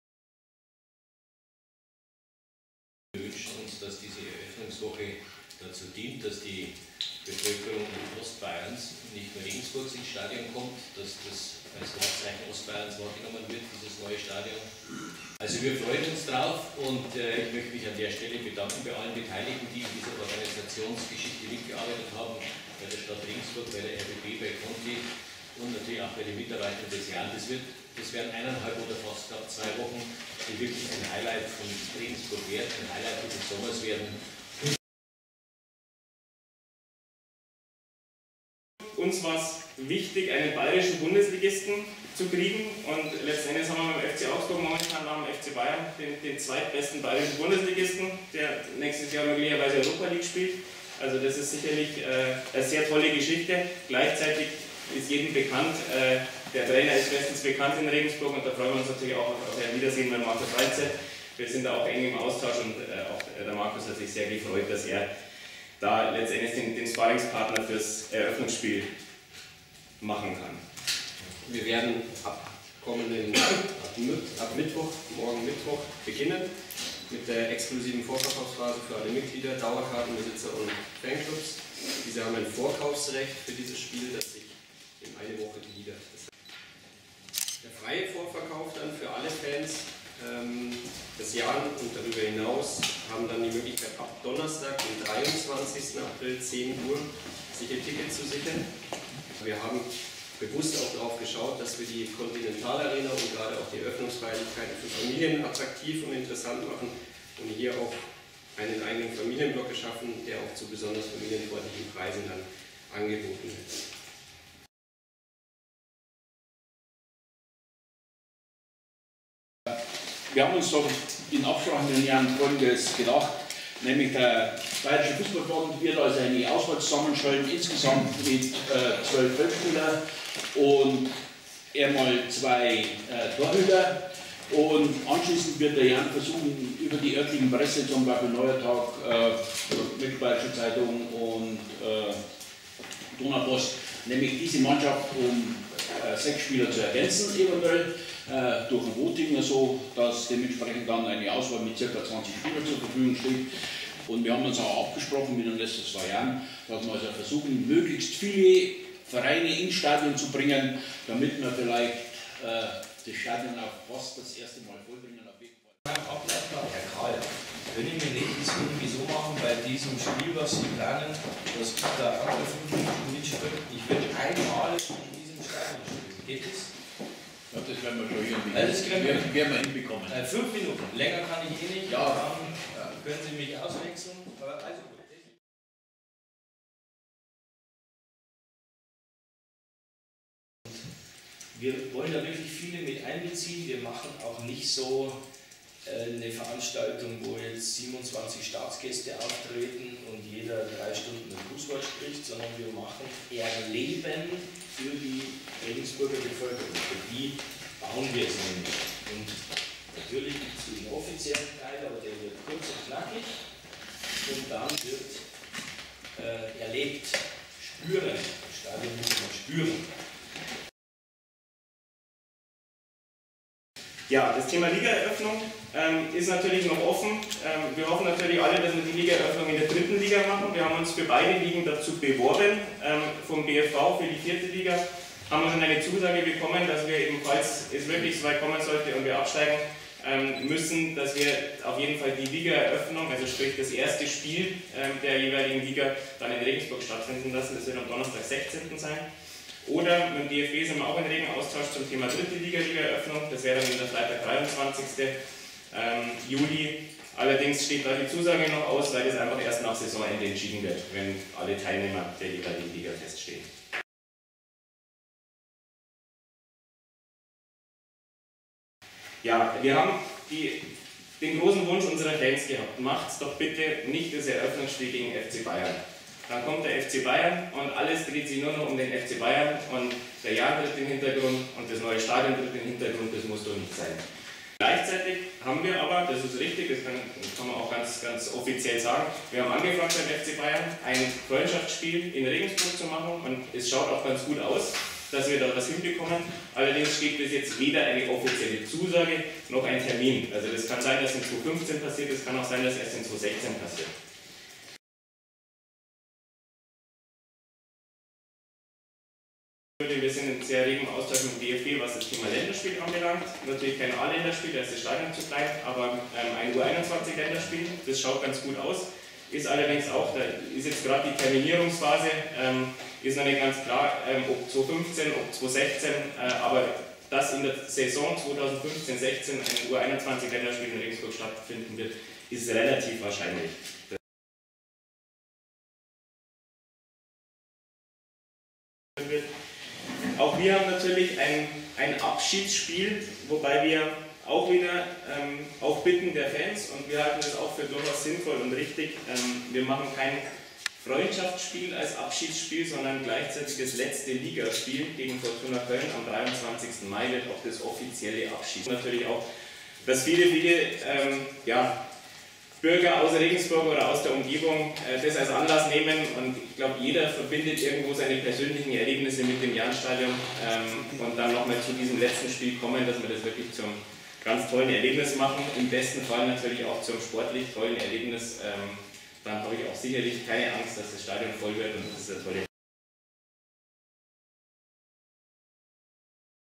Wichtigste Aussagen Pressekonferenz Eröffnung Continental Arena (14.04.2015)
Heute gab der SSV Jahn gemeinsam mit der Stadt Regensburg, dem Regiebetrieb Arena Regensburg und dem Bayerischen Fußball-Verband die Planungen zur Eröffnung der Continental Arena bekannt. Am 10. Juli kommt der FC Augsburg zum offiziellen Eröffnungsspiel. Doch es stehen noch einige weitere Veranstaltungen auf dem Plan.